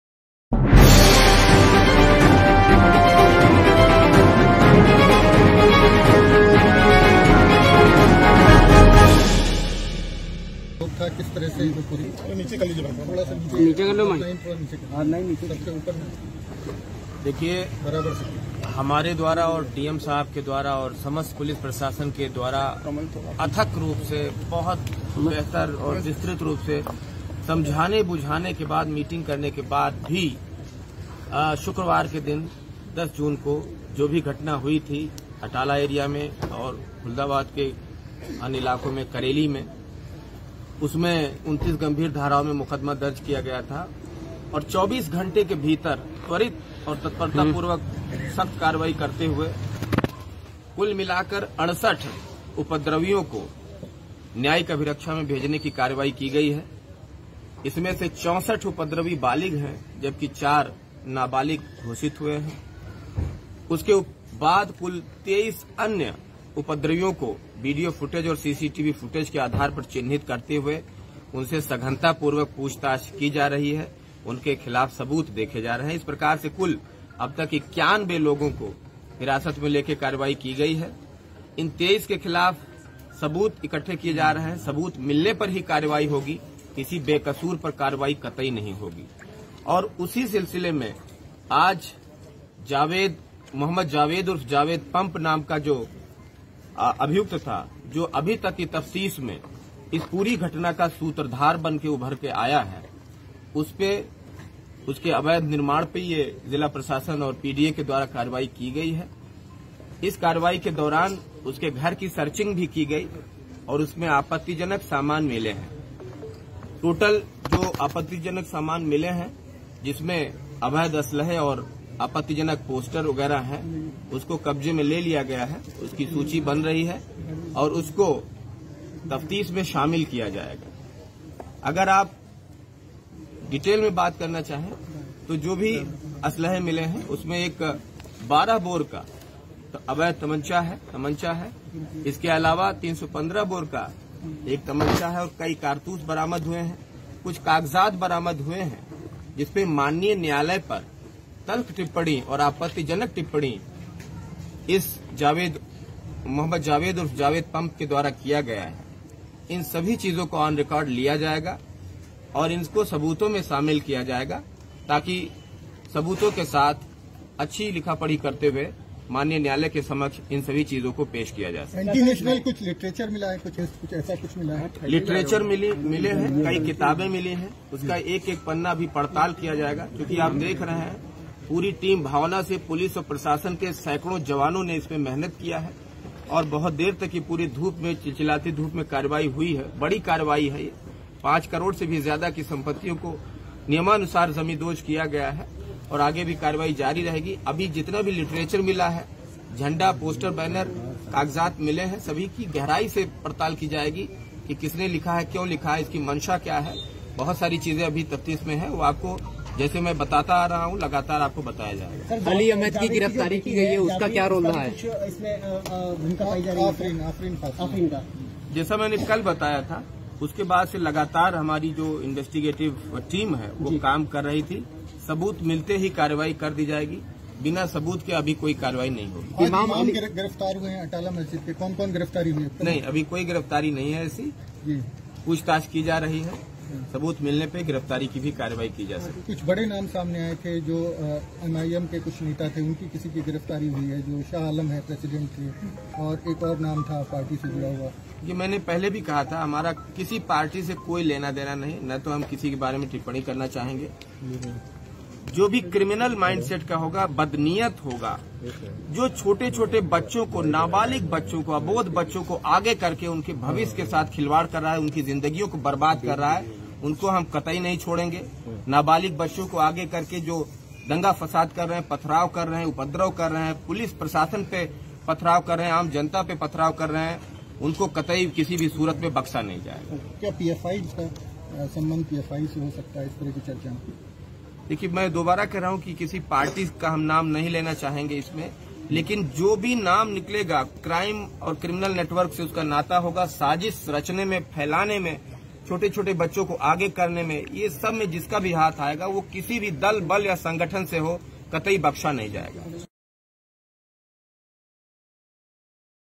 जावेद पंप के मकान का PDA द्वारा गिराए जाने के सम्बन्ध में SSP प्रयागराज द्वारा दी गयी बाइट
दिनांक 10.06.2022 को हुई घटना के सम्बन्ध में अब तक की गयी पुलिस कार्यवाही व घटना के मुख्य सूत्रधार जावेद मोहम्मद उर्फ जावेद पंप के अवैध निर्माण के विरुद्ध जिला प्रशासन/PDA द्वारा की गयी कार्यवाही के सम्बन्ध में वरिष्ठ पुलिस अधीक्षक प्रयागराज द्वारा दी गयी बाइटः-